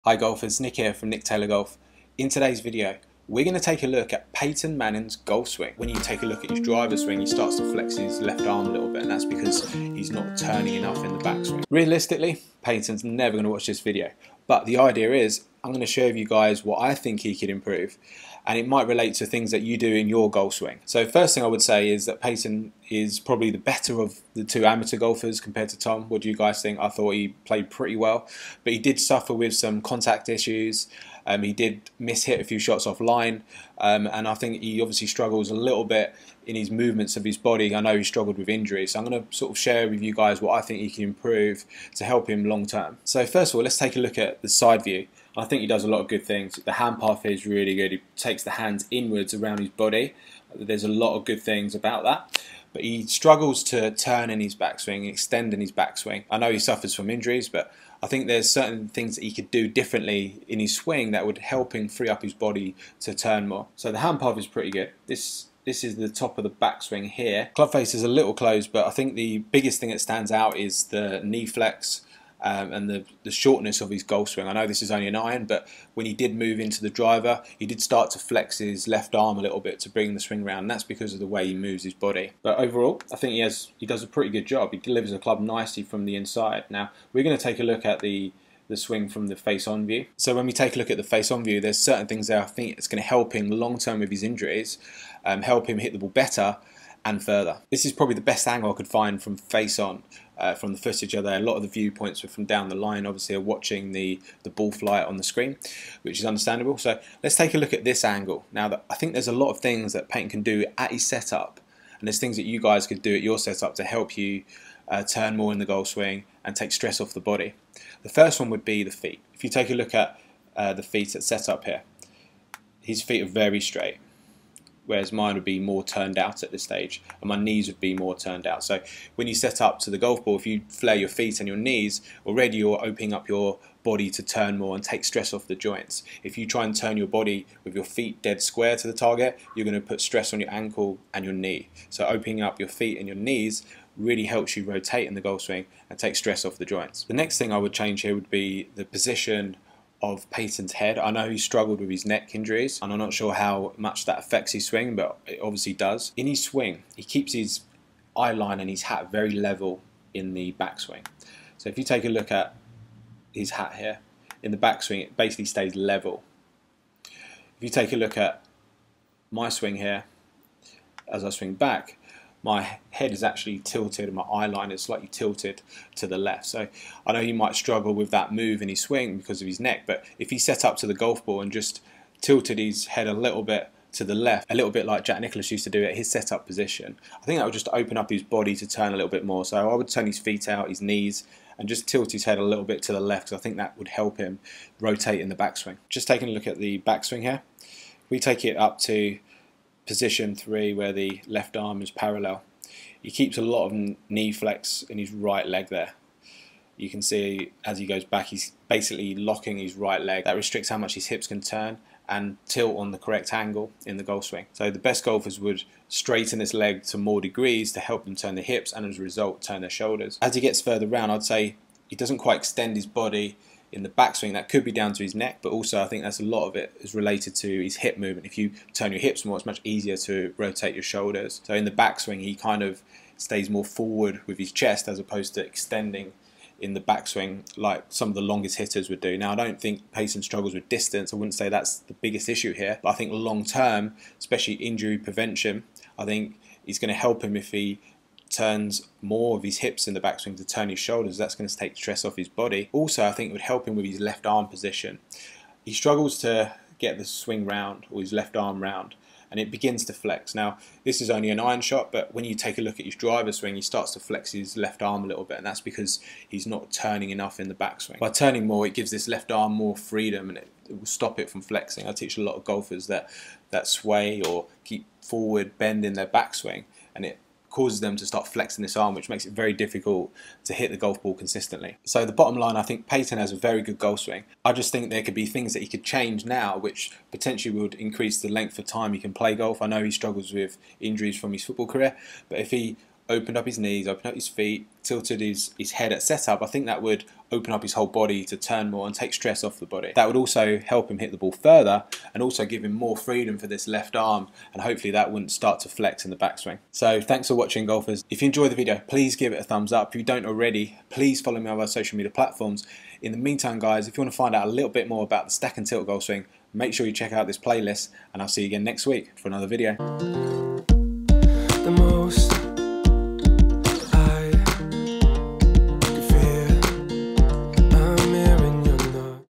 Peyton Manning Golf Lesson
Peyton Manning and Tiger Woods teed it up at the weekend against Tom Brady and Phil Mickelson in The Match. In this video Nick analyses Peyton's golf swing to help him hit the ball better, further and potentially reduce the risk of injury in the long term.

🎥 ONLINE GOLF LESSONS WITH NICK via SKILLEST app (top 15 coach over 400+).